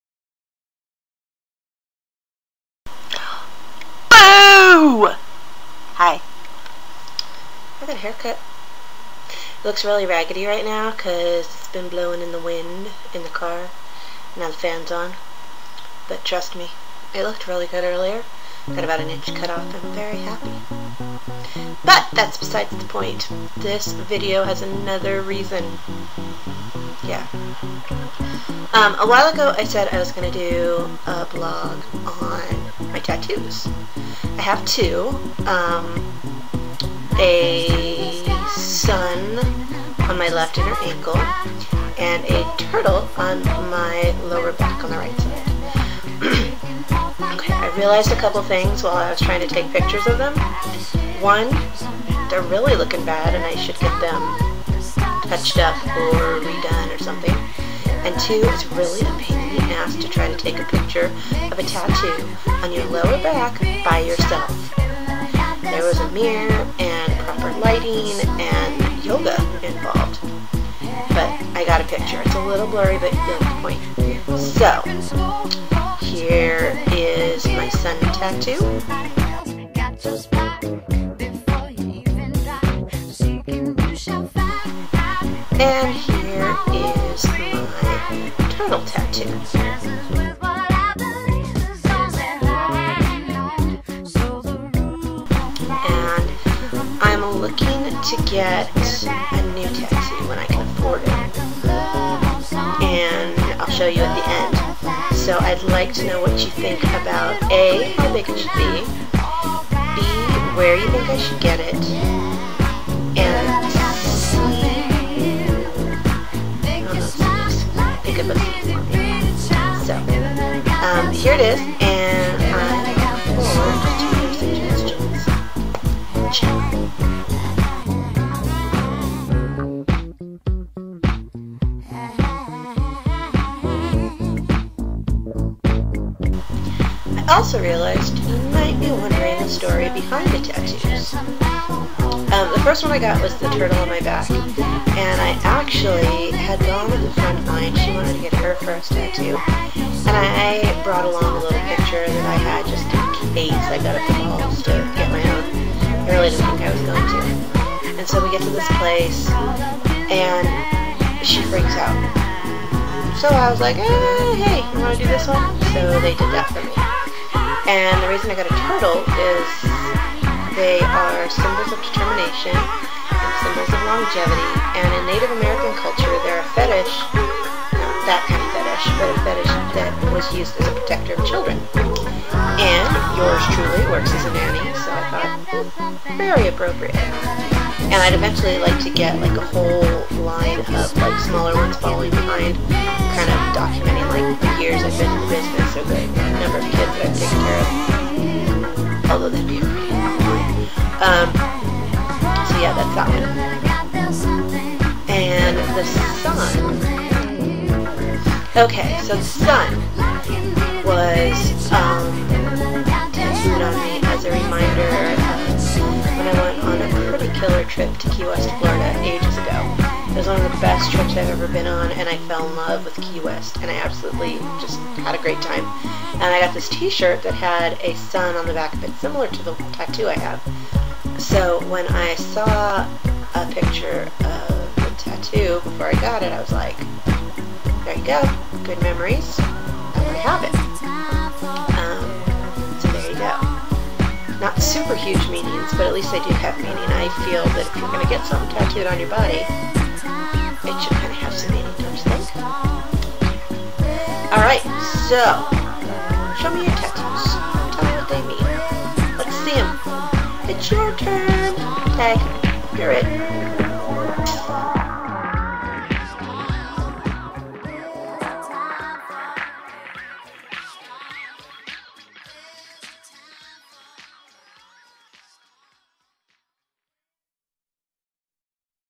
me and my tattoos
just a little history behind the tats i have and the one i'd like to get. need your opinion on where i should put it and how big.